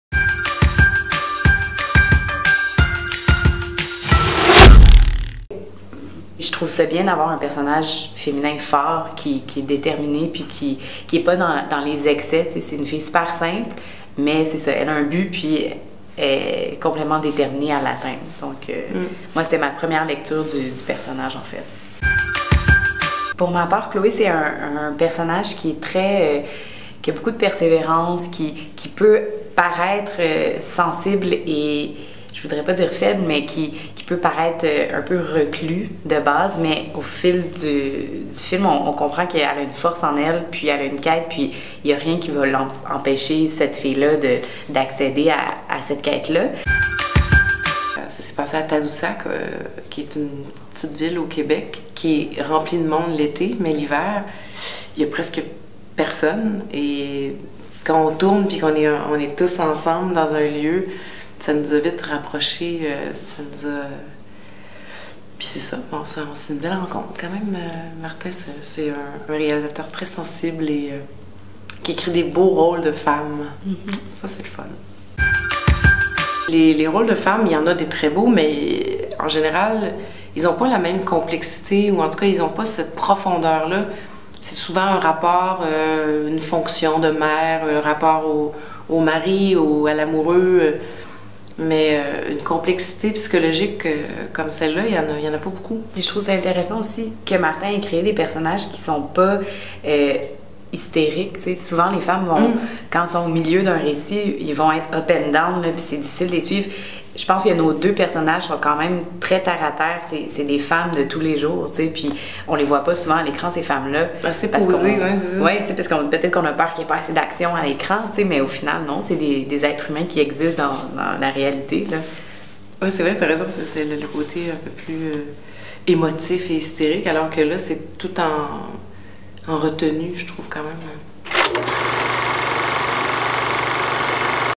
Isabelle Blais et Camille Mongeau - Tadoussac | L'Interview | FIFF 2017
Interview d'Isabelle Blais et Camille Mongeau, comédienne de "Tadoussac".

"Tadoussac" est sélectionné en Compétition Officielle au FIFF.